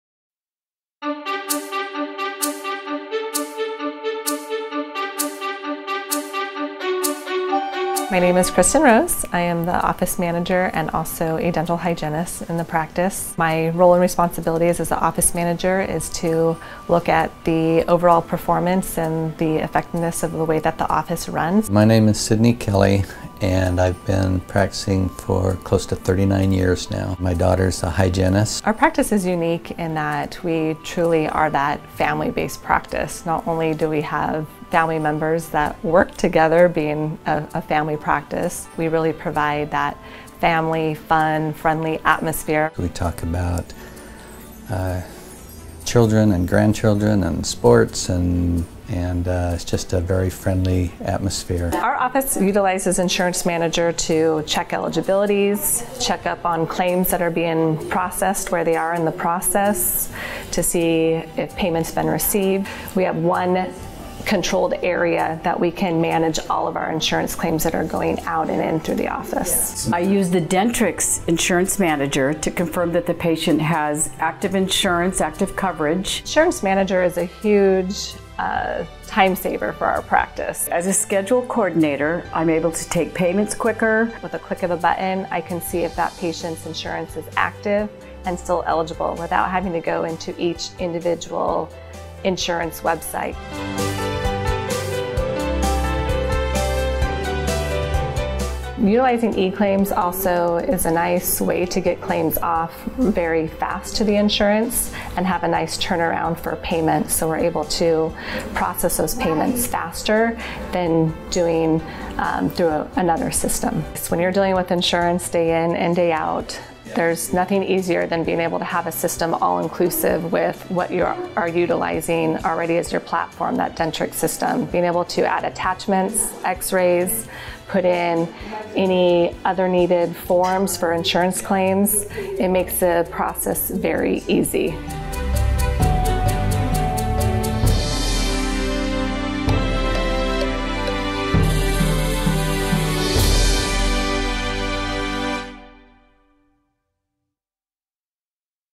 Dr. Sidney Kelly - Quicker Reimbursements Using Dentrix Insurance Manager and eClaims
The staff at Dr. Kelly's office enjoy a single, controlled area to manage all of their insurance claim submissions with Dentrix Insurance Manager. Integrated directly in Dentrix, eClaims provides them a simple, pain-free insurance claims processing workflow. They have eliminated the need to switch back and forth between programs when submitting claims. The benefits they are realizing include quicker claim process and payment turnaround.